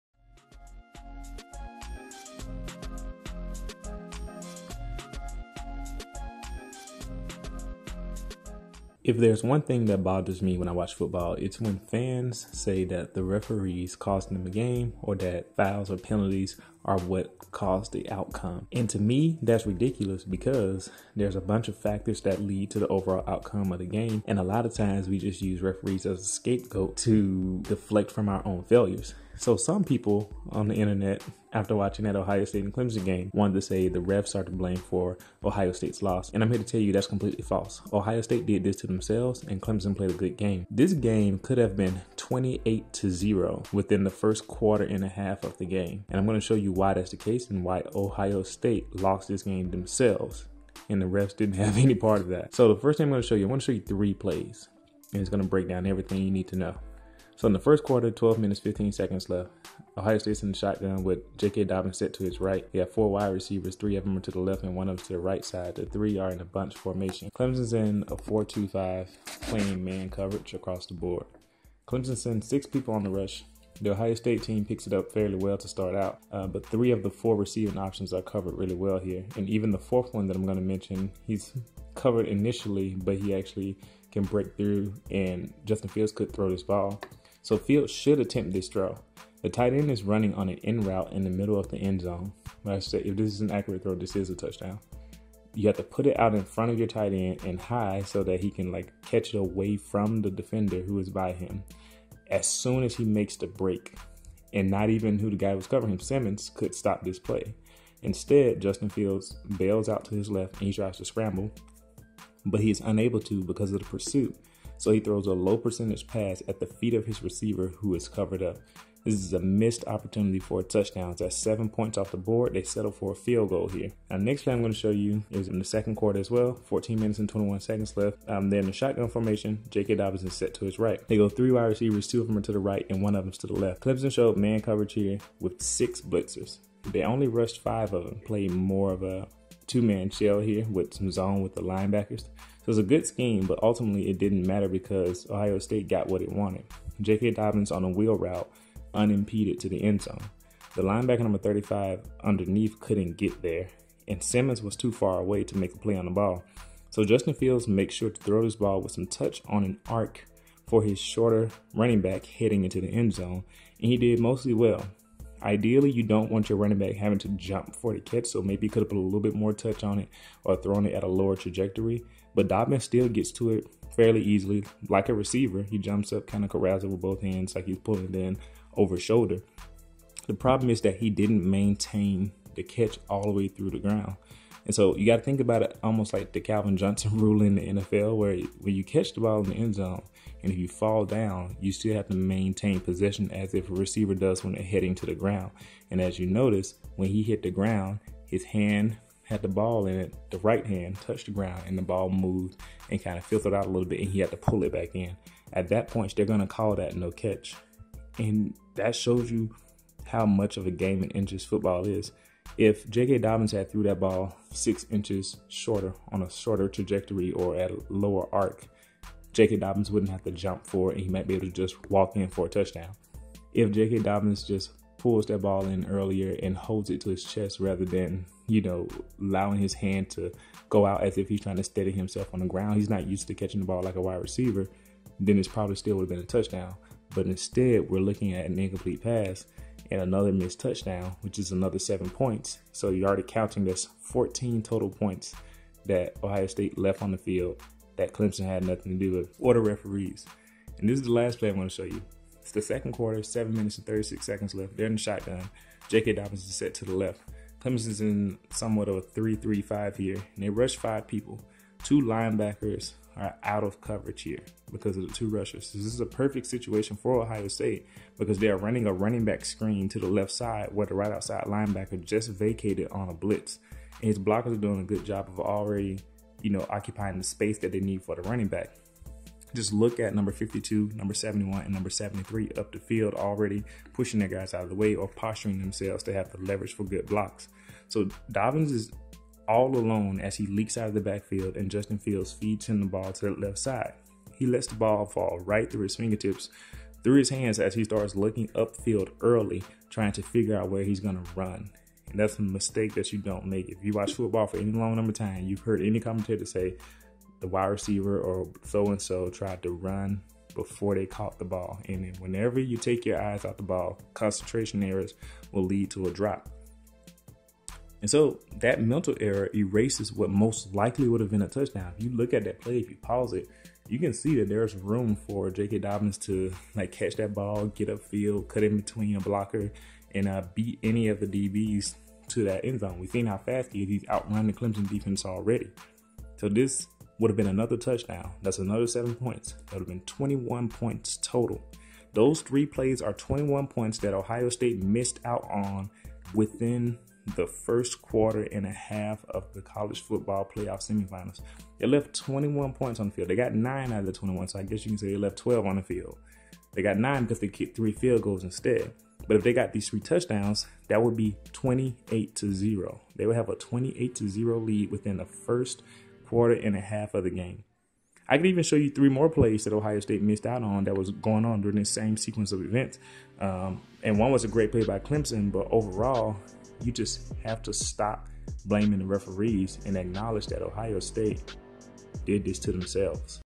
Ohio State CAN'T Blame the Refs for their Loss!!
I'm here to tell you why Ohio State can't blame their loss on the refs. They had plenty of opportunities to blow this game open in the 1st quarter and a half and failed to capitalize.